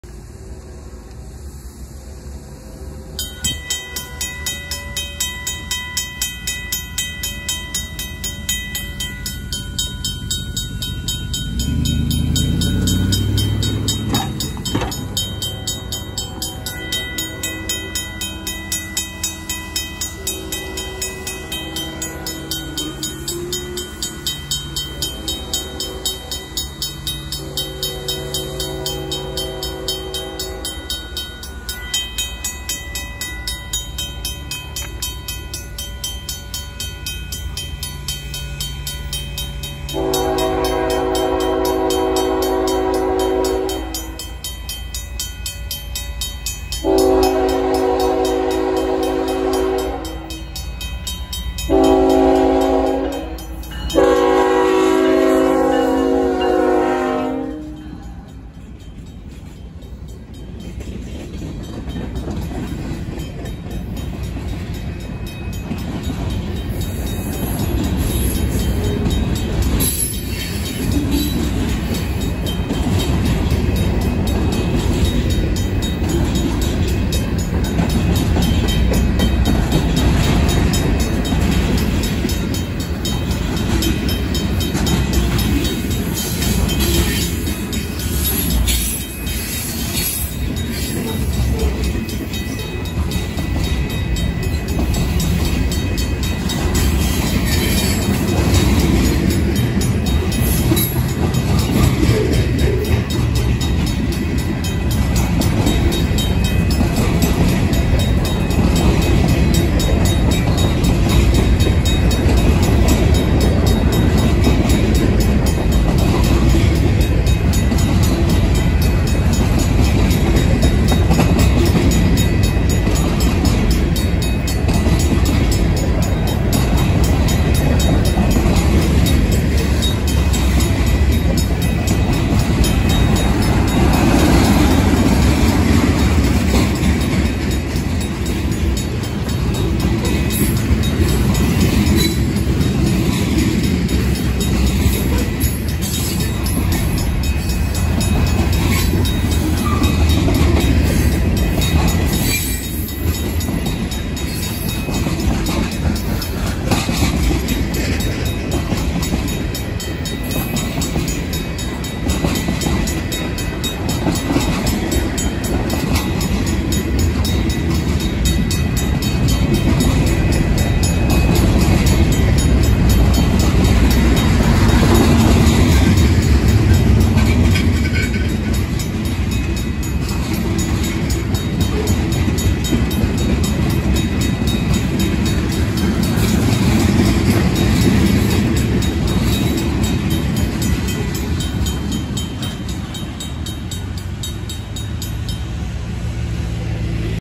Emerald Street railroad crossing, UP 2630 Manifest, Onawa, IA
A Union Pacific mixed freight led by one locomotive heads north through Onawa.
Crossing info
Owner and maintainer: Union Pacific Railroad
Location: Onawa, Iowa
Line: Union Pacific Sioux City Subdivision
Left signal
Lights: 2 pairs of Safetran 12x24 inch with GE Dotted LEDs
Right signal
Bell: RACO mechanical bell